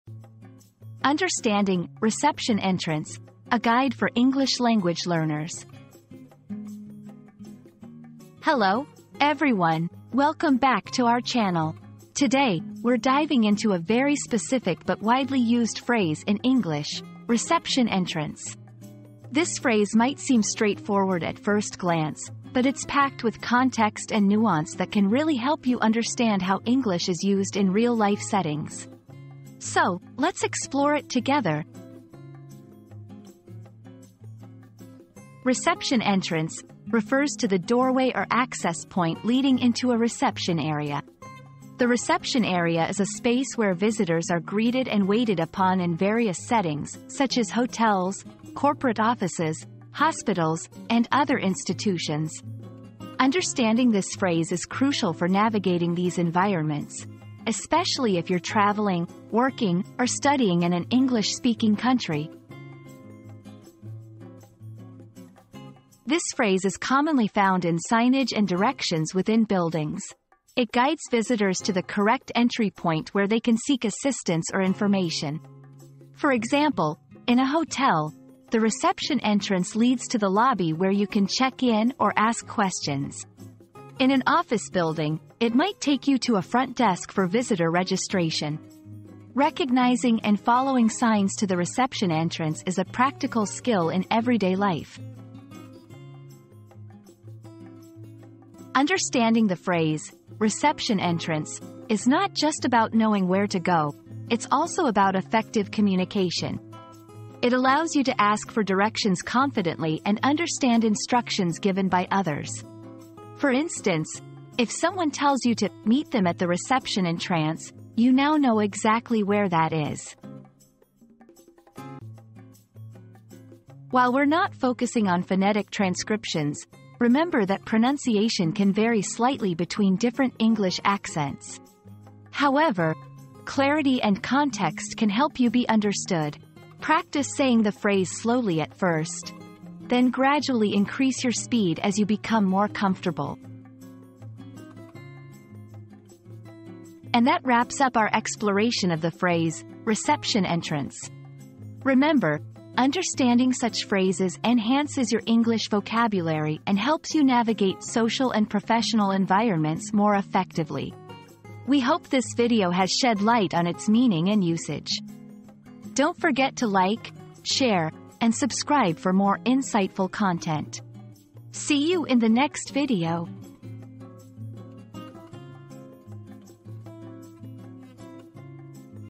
Understanding "Reception Entrance": A Guide for English Language Learners
00:00 • Introduction - Understanding "Reception Entrance": A Guide for English Language Learners
00:34 • Breaking Down the Phrase
01:05 • Usage and Context
01:38 • Importance in Communication
02:03 • Pronunciation Tips